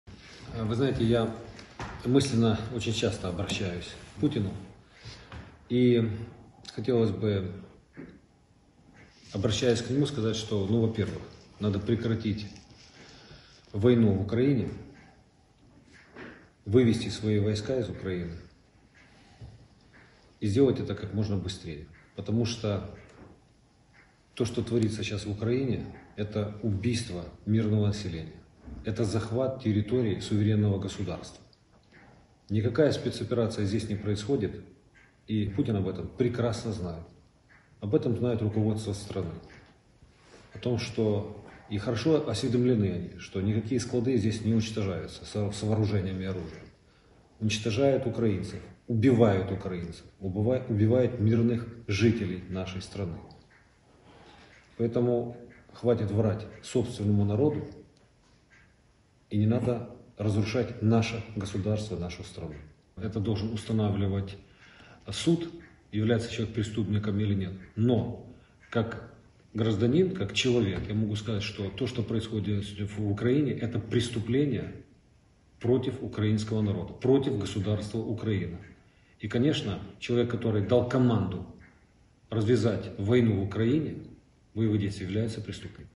Те, що відбувається в Україні – це злочин проти українського народу, проти держави Україна
«Звертаючись до путіна, я сказав би: «Досить брехати своєму народові! Досить руйнувати нашу країну – Україну!», сказав мер Одеси в інтерв'ю австрійському телеканалу RAI-Tv ORF TV.
Одеський міський голова вкотре наголосив, що в Україні ведеться війна, відбувається захоплення території суверенної держави, і не йдеться про жодну спеціальну операцію. За словами мера Одеси, керівництво країни-агресора добре обізнані, що б'ють не за складами зі зброєю, а мирним населенням.
«Суд має встановлювати, чи є людина злочинцем чи ні. Але як громадянин, як людина, я можу сказати: те, що відбувається в Україні – це злочин проти українського народу, проти держави Україна», – сказав Геннадій Труханов.